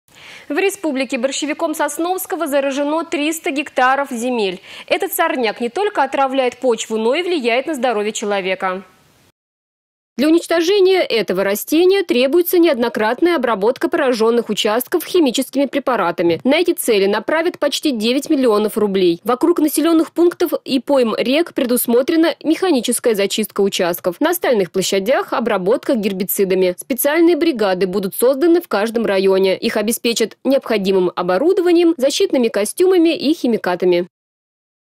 В Чувашии выделены средства на борьбу с борщевиком
В республике борщевиком Сосновского заражено 300 гектаров земель. Этот сорняк не только отравляет почву, но и влияет на здоровье  человека.